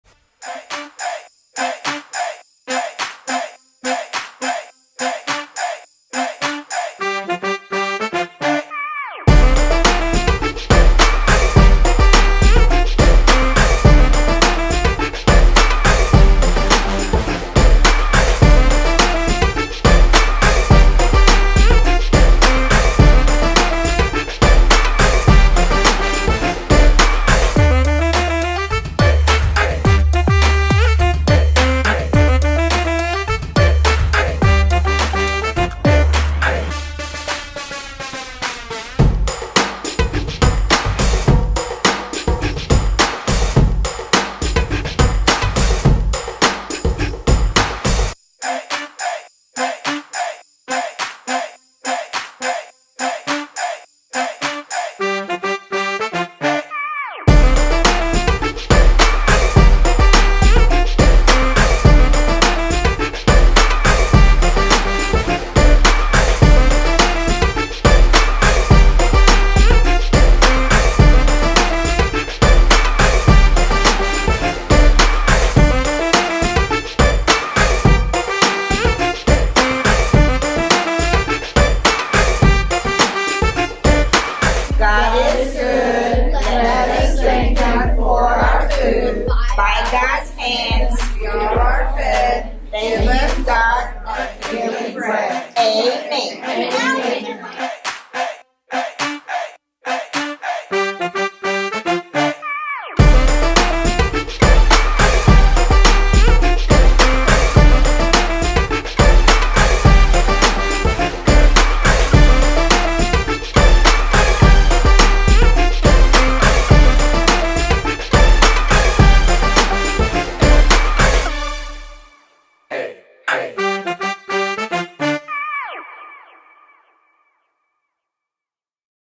Food Truck Party VBS at NVC! 2023 Recap Video
We wrapped up our summer break with a fun-packed VBS called Food Truck Party. See the recap video for just a glimpse of the week we had.